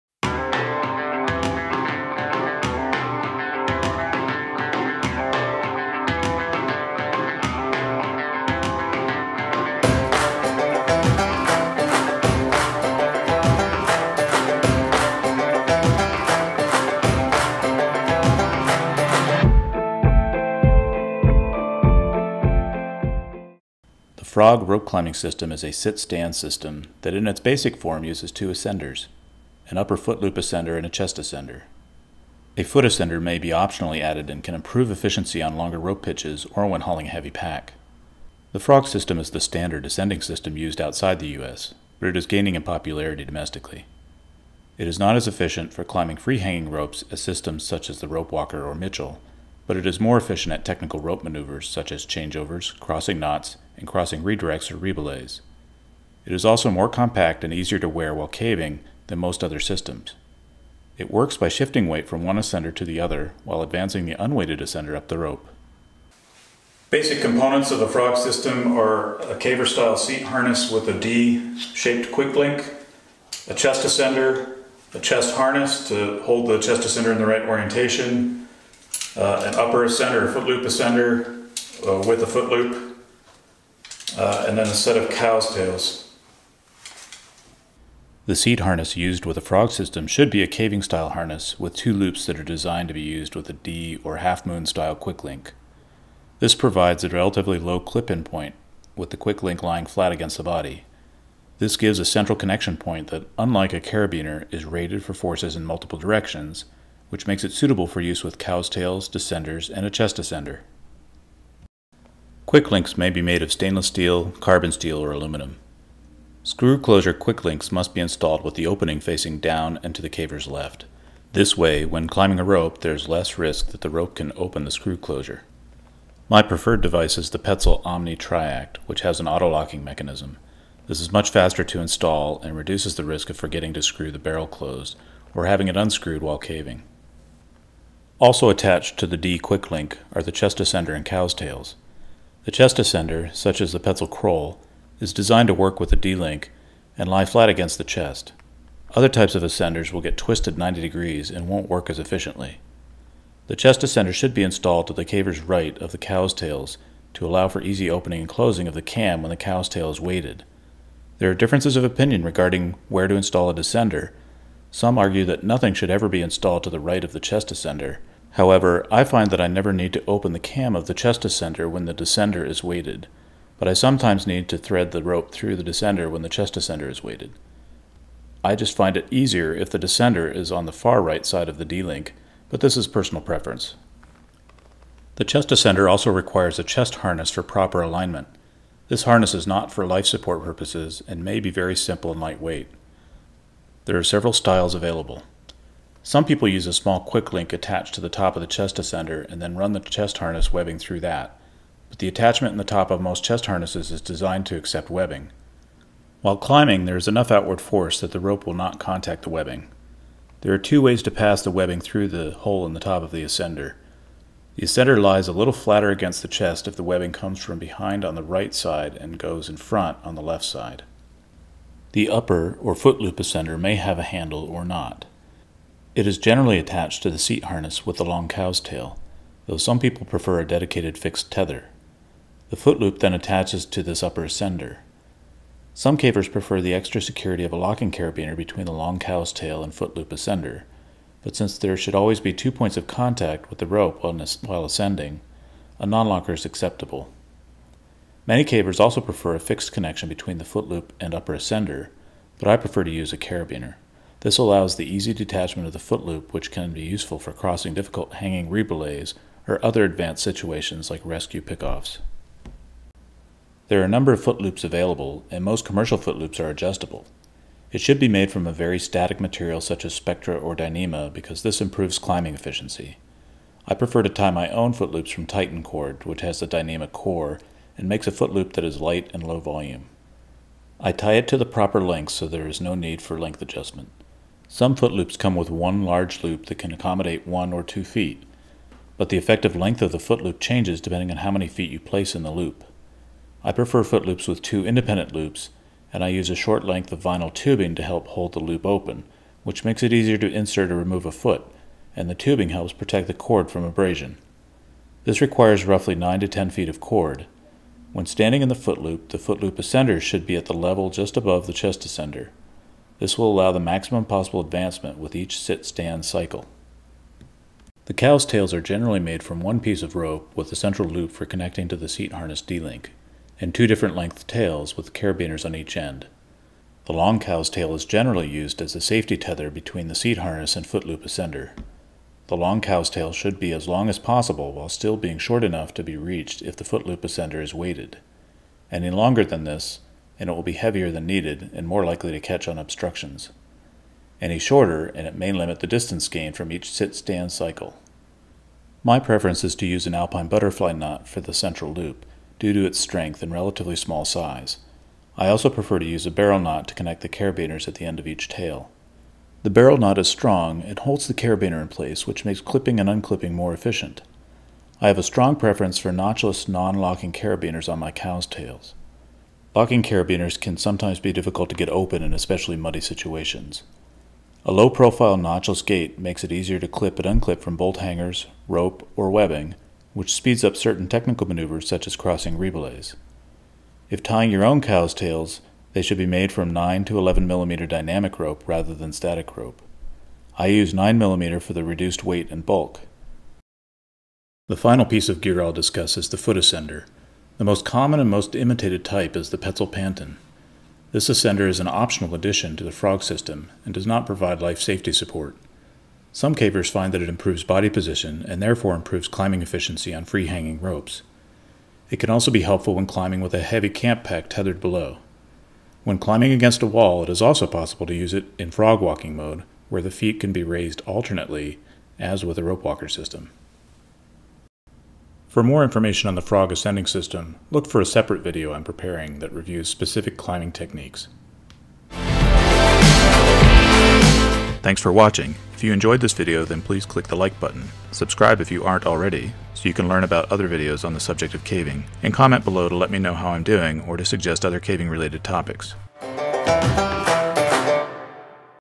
Frog Ascending System for Caving - An Overview
Caving Vlog #32
An overview of the frog ascending system.